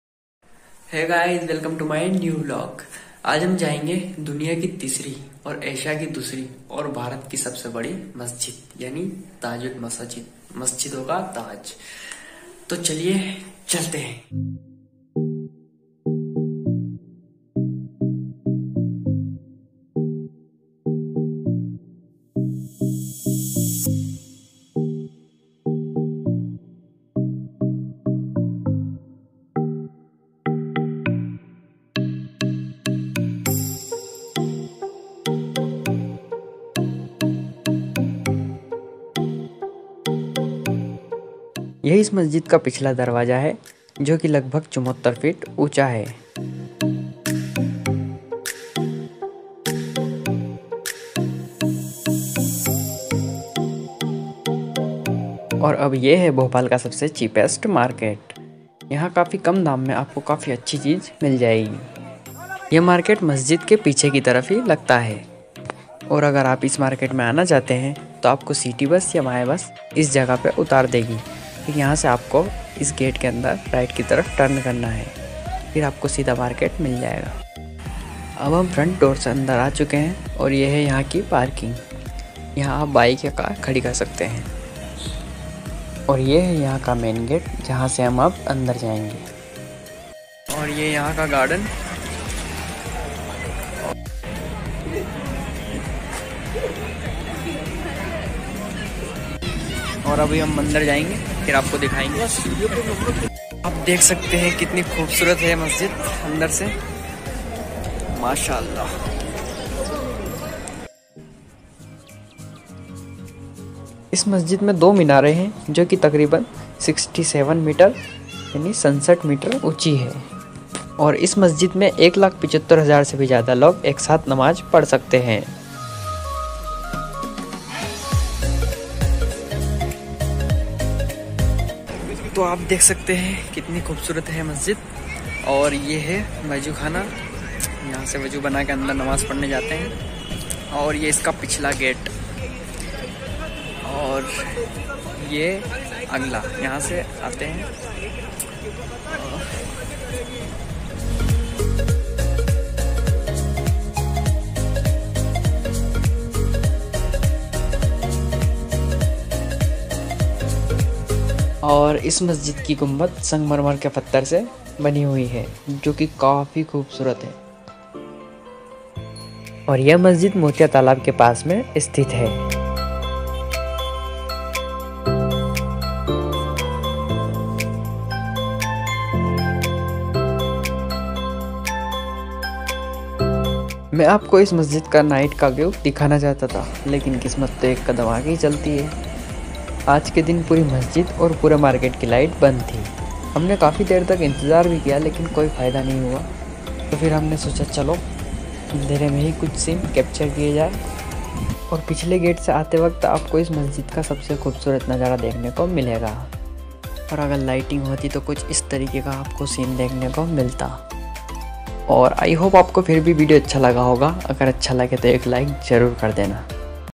TAJ UL MASAJID BHOPAL VLOG Biggest Mosque in India, Vlog #3, VLOGGER_2.0
Taj ul masajid bhopal

Biggest Masjid in India

Cheapest market Bhopal...